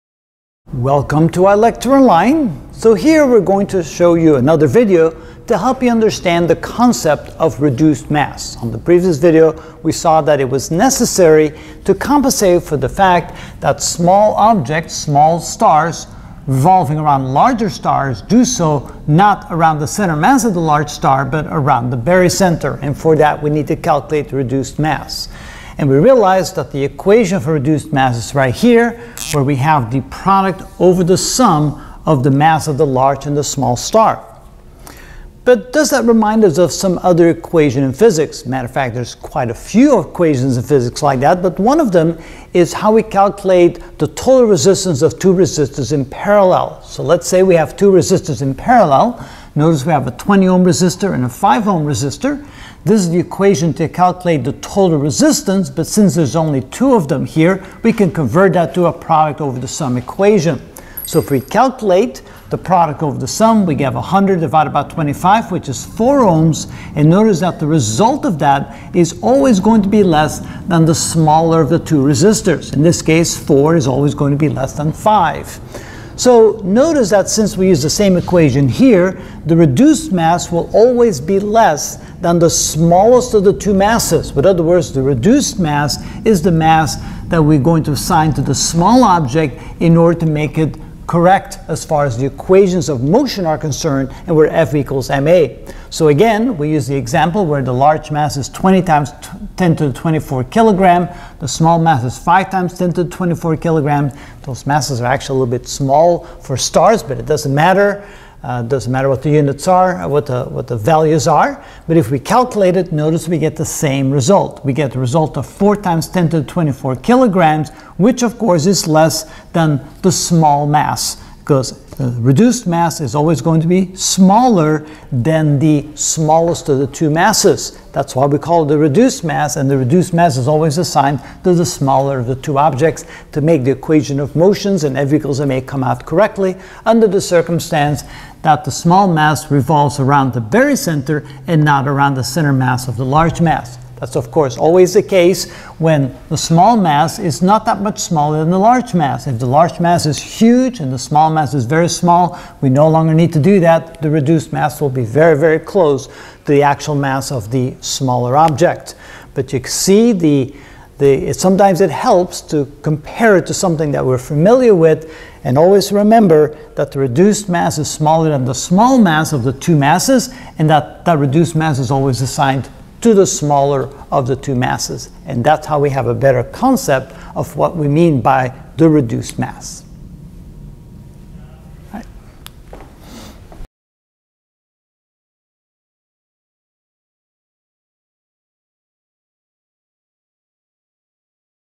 Astrophysics: Binary Star System (32 of TBD) What is Reduced Mass? "Product Over the Sum"
We are going to learn the concept of reduced mass. We notice the equation of reduced mass reminds us of the equation of how to calculate the total resistance of 2 resistors in parallel.